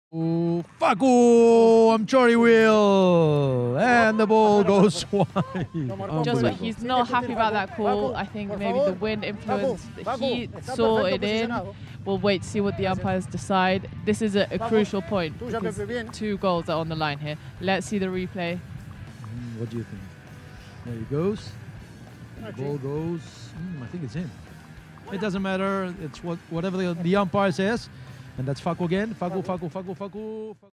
🤪 Facundo Pieres gets angry! | Ellerstina vs. La Aguada | Xtreme Polo League - XPL
👨‍⚖️ Whilst taking on La Aguada, Ellerstina captain complained when the goal judge did not validate his penalty shot, which was worth double.
🗯️ "No! No! No!", Pieres shouted at the umpire, who told him that his fellow umpire was in the right location to make the call.
🎤 We were able to hear the whole exchange due to the microphones worn by XPL umpires.